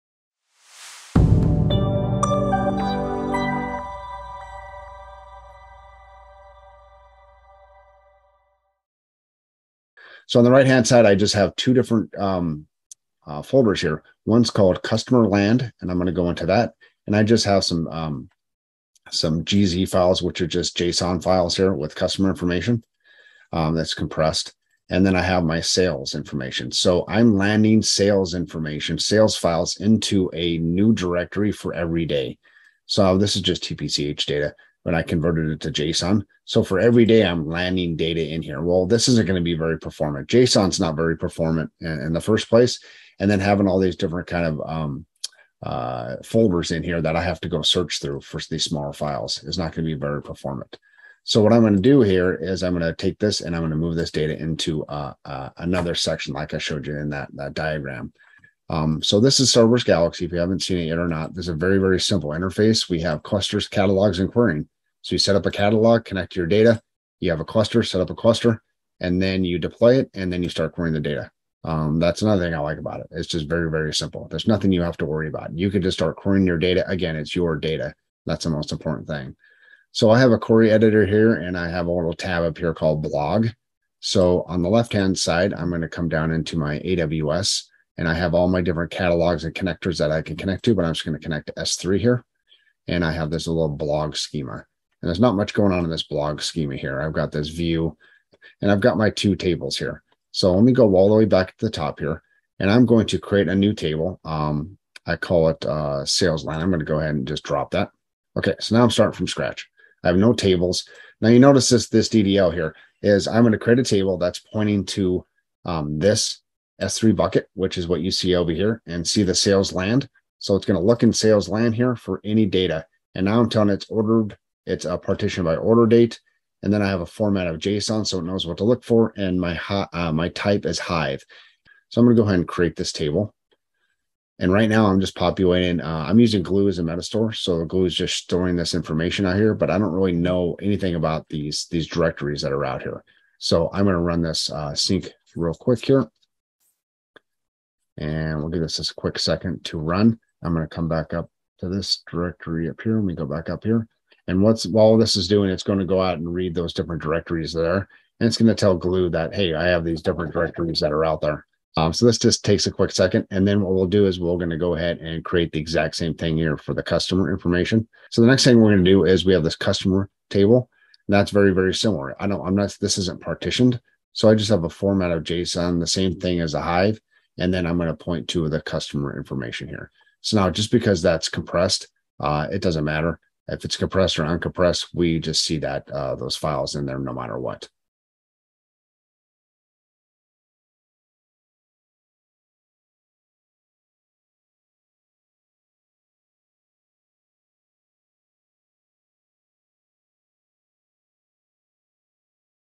Setting up your Starburst Galaxy data lakehouse | Starburst Academy
Excited to learn more about data lakehouses using Apache Iceberg? Starburst Academy has a whole course on the topic

If you want to get hands-on with Starburst, get up and running in seconds with our Starburst Galaxy: Getting started tutorial

In this walkthrough, we demonstrate how to optimize your data storage for performance and efficiency.

We start by exploring the challenges of storing JSON files directly in S3 and show why it's not the most performant solution. Then, we introduce Starburst Galaxy, a user-friendly interface for managing clusters, catalogs, and querying data.

Using Starburst Galaxy, we create tables to organize our data stored in AWS S3 buckets. We demonstrate how to define tables for both sales and customer information, specifying partitioning and file formats for efficient querying.

Join us as we streamline data storage and access, ensuring your data lakehouse is set up for success from the ground up! Don't miss out on optimizing your data infrastructure with Starburst Galaxy.